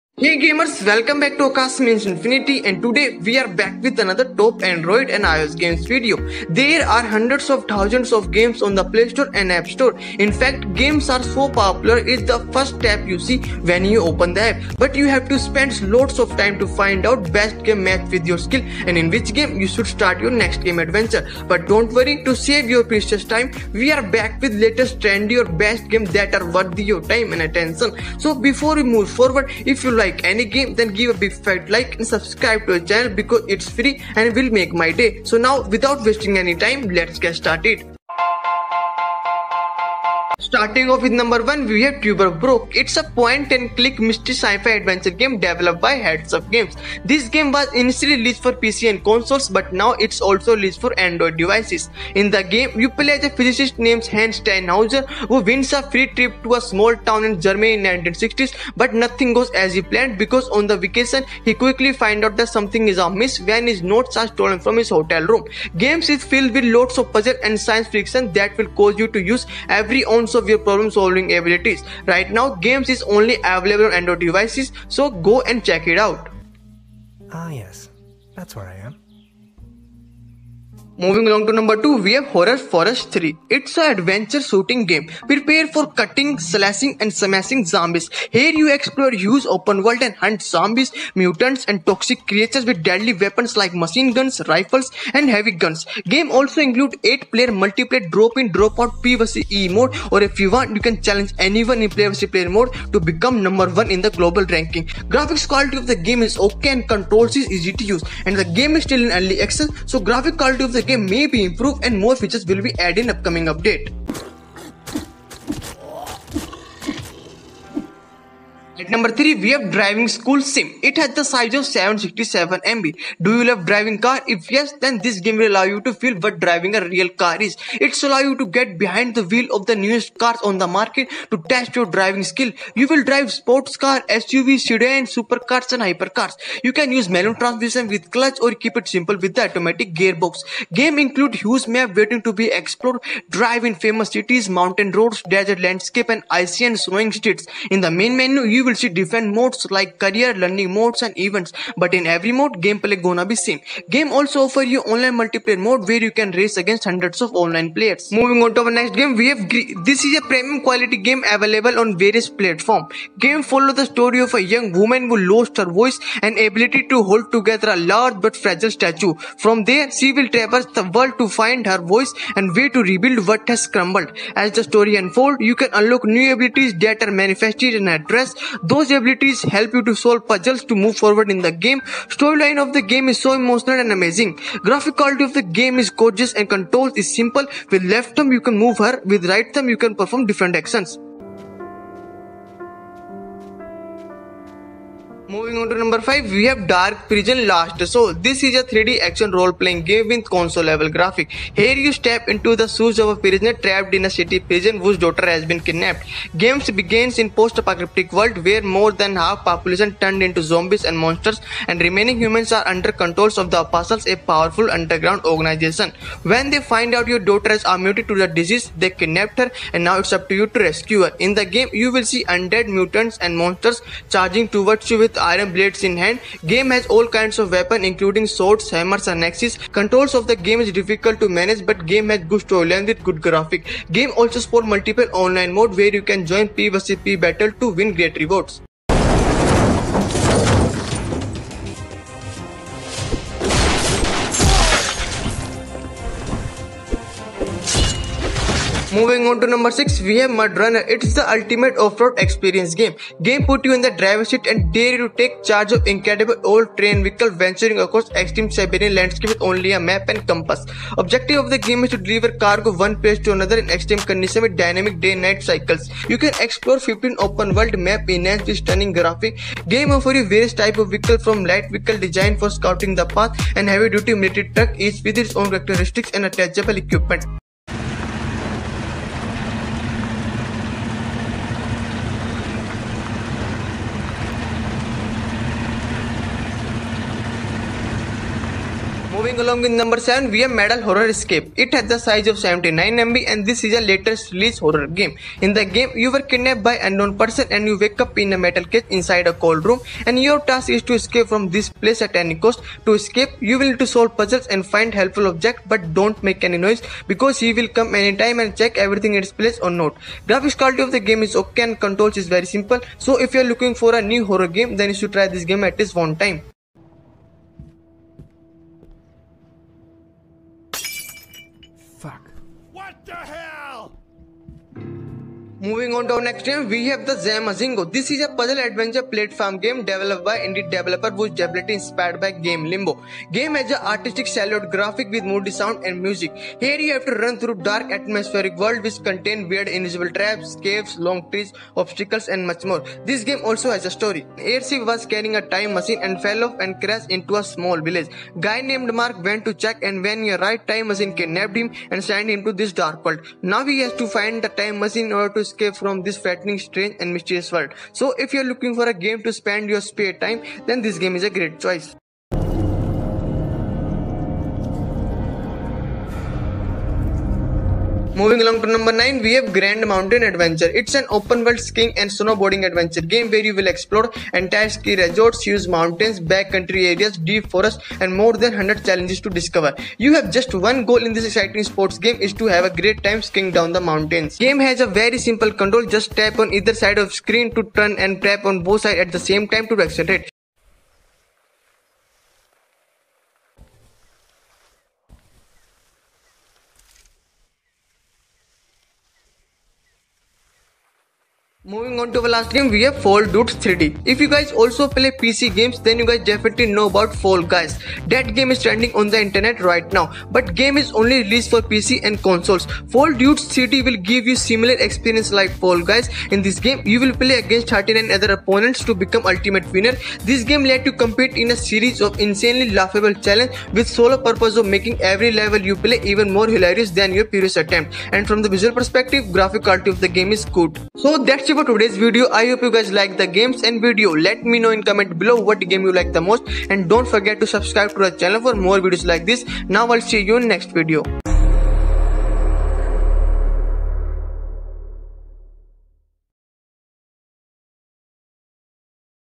TOP 10 ANDROID & IOS GAMES IN September 2020 ||HIGH GRAPHICS|| LATEST RELEASE ||(Online/Offline)NEW
HELLO GAMERS MY NAME IS AKASH SAINI , TODAY IN THIS VIDEO I WILL SHOW YOU TOP 10  ANDIROD GAMES YOU SHOULD PLAY IN SEPTEMBER 2020. I HOPE YOU LIKE THE GAME.

MY DETAILS

NAME - AKASH SAINI
AGE - 21 YEARS
DEVICE ( EDITING) - NOTE 5 PRO